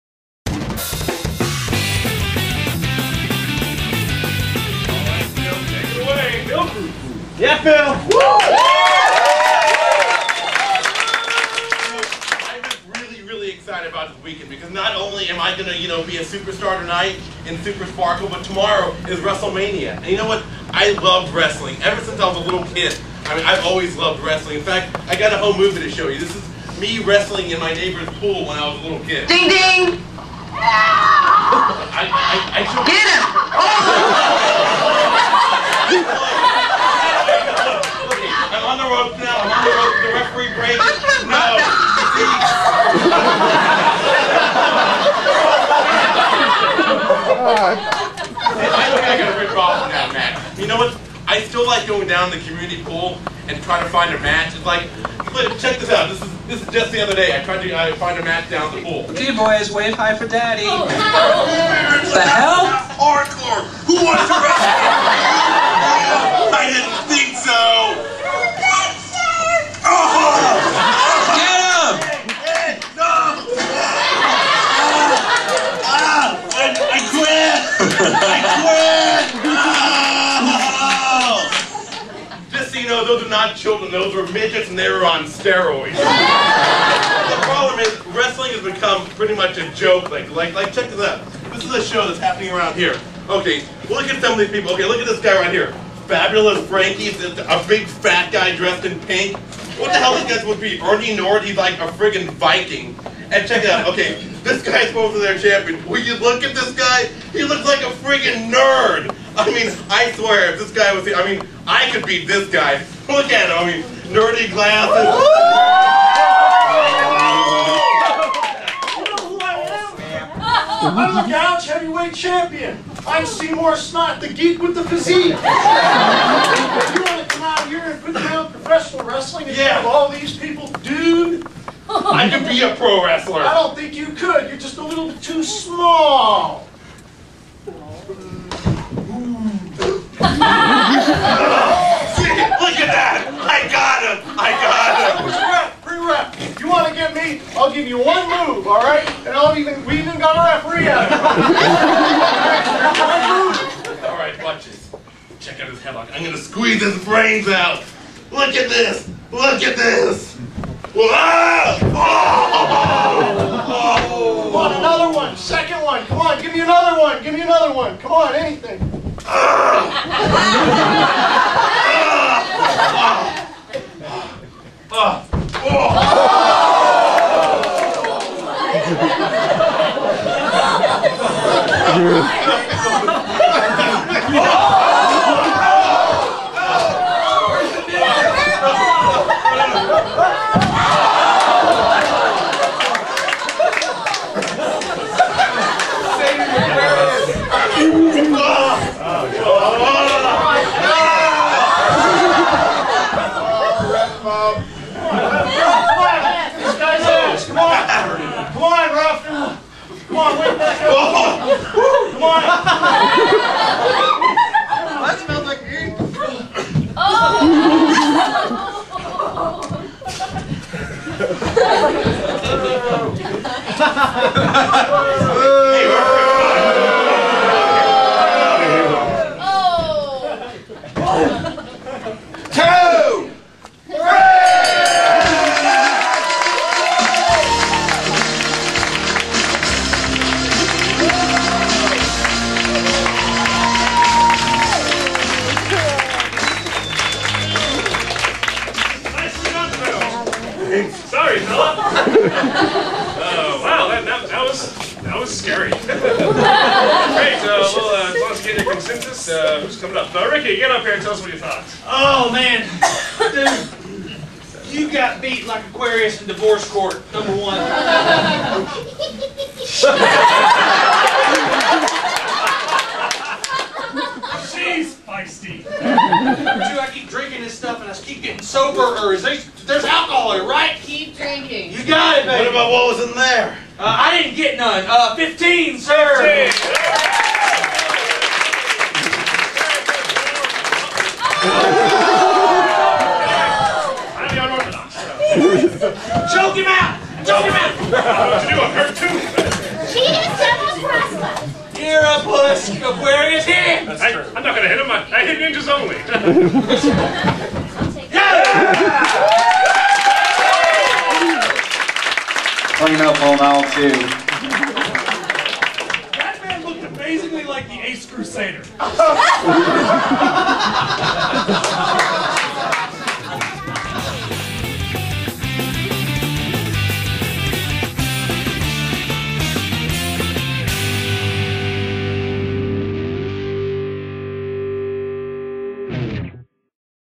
SEASON 2 - WEEK 6 - Phil
In his week six performance, contestant PHIL KRUH (and partners Cary LaMay and Seymour Snott) explain how his love of wrestling went all the way back to his days of the kiddie pool.